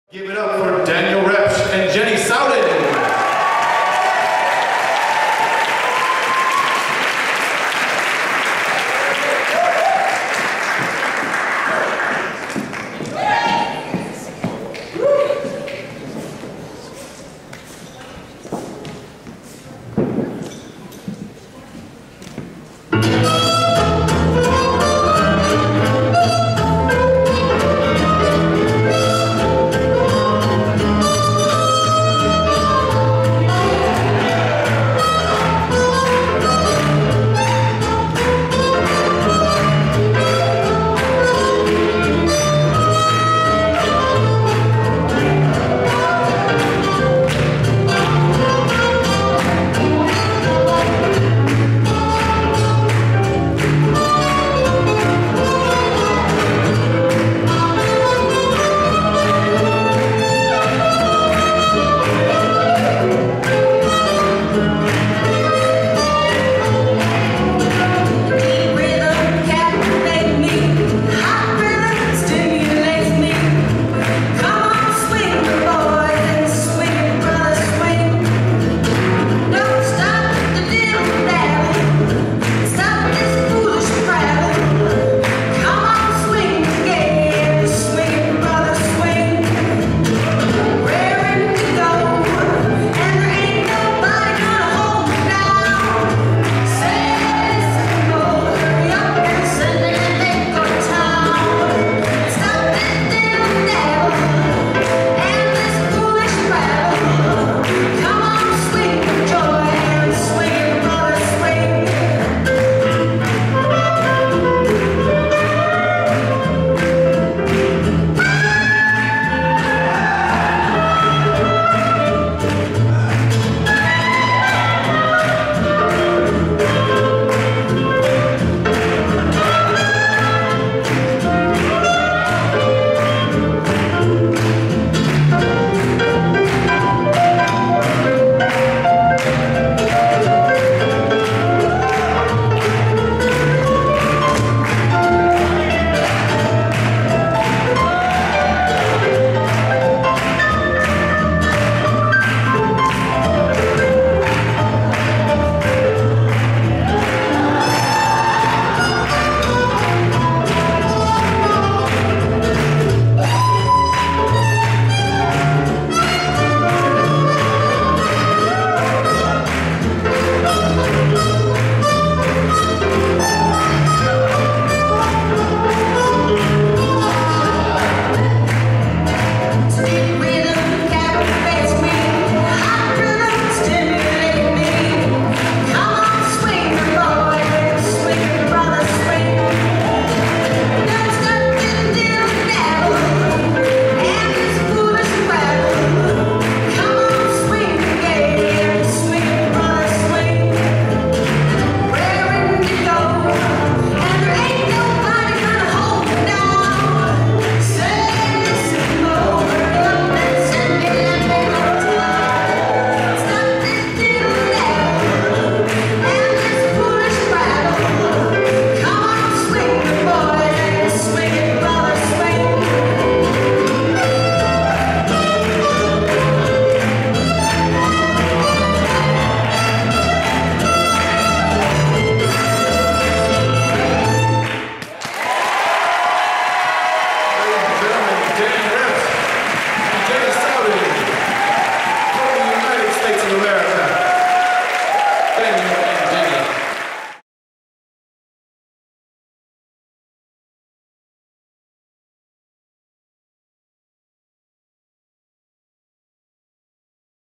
ILHC 2015 - Open Showcase Finals - Daniel Repsch & Jenny Sowden (US)
The International Lindy Hop Championships ILHC 2015
Open Showcase Finals
Daniel Repsch & Jenny Sowden (US)